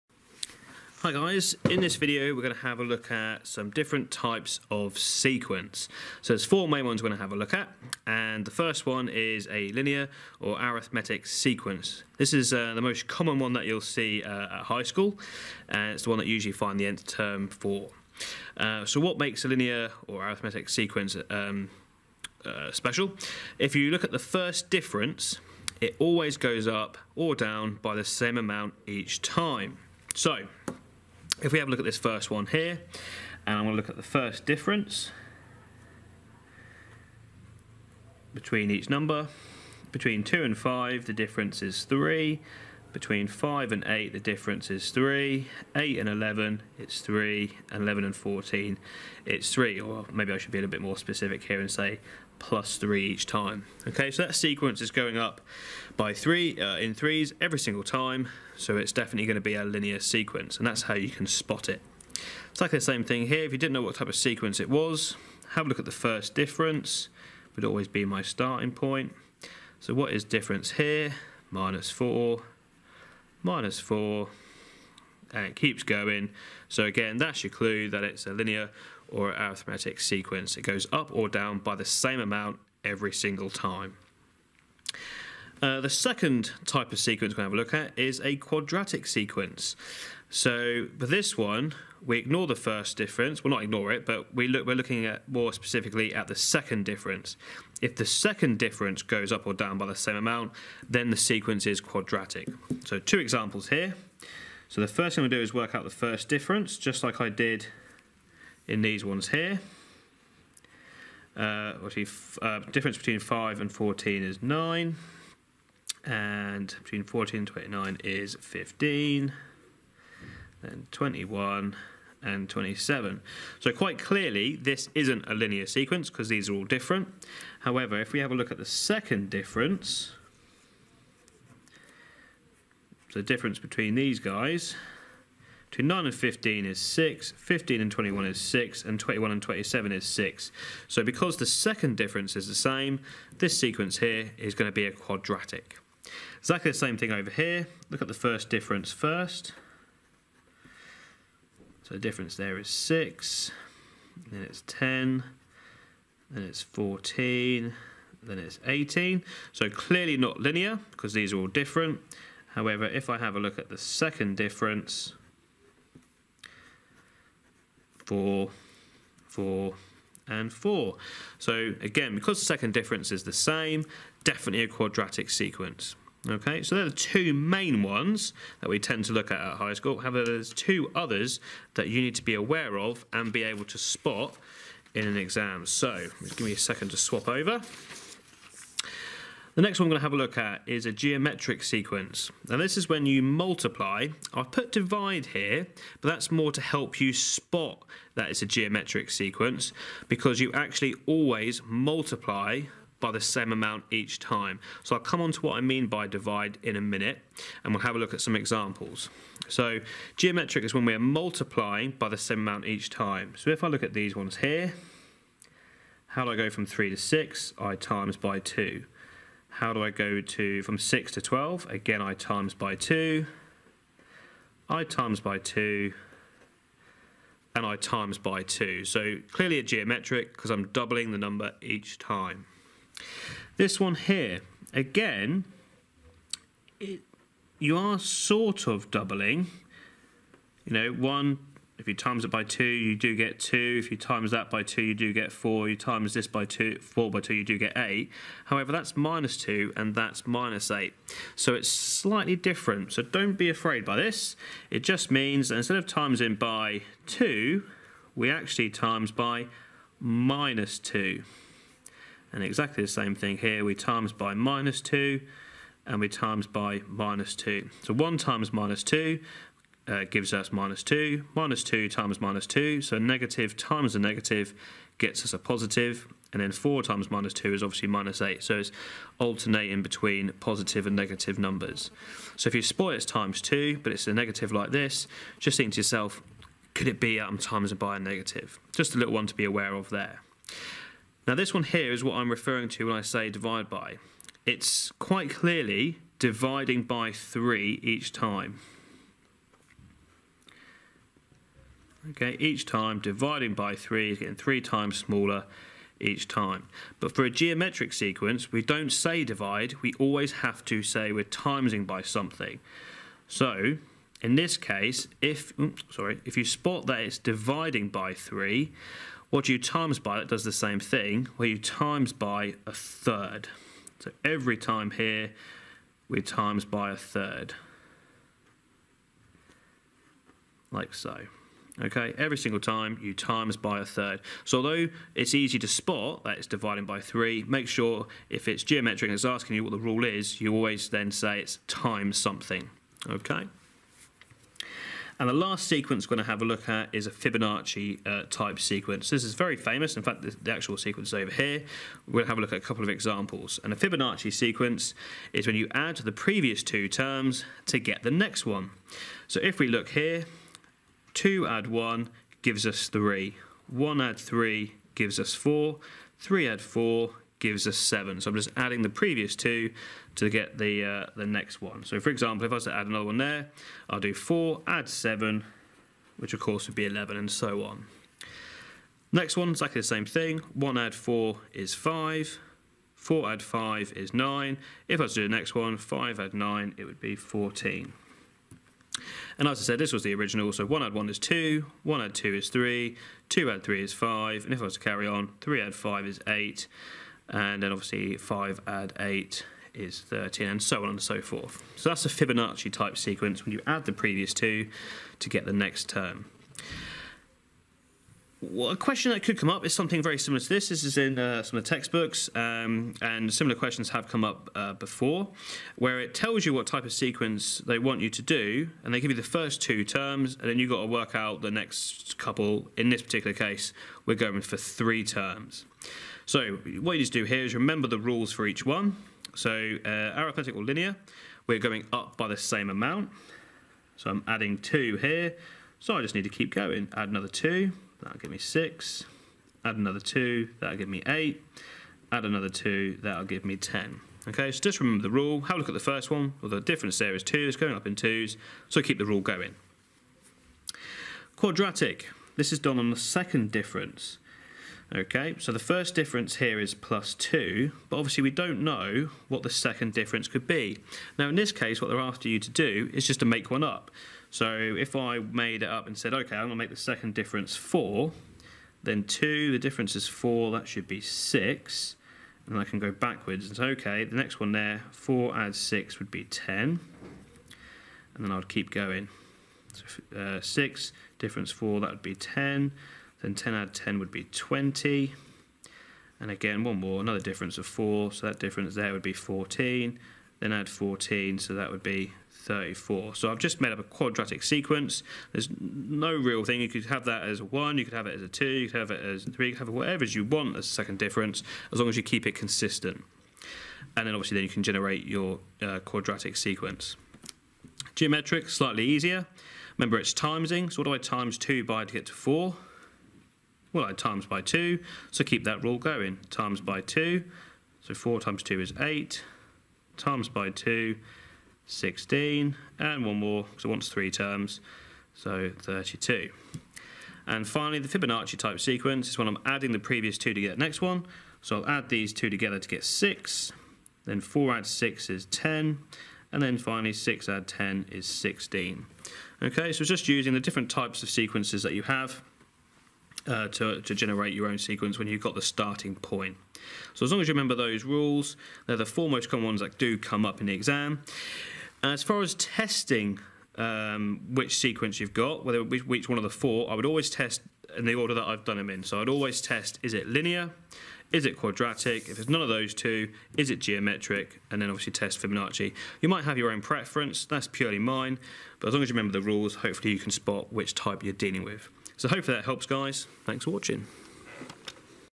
Types of Sequence
Examples of linear, quadratic, geometric and Fibonacci type sequences.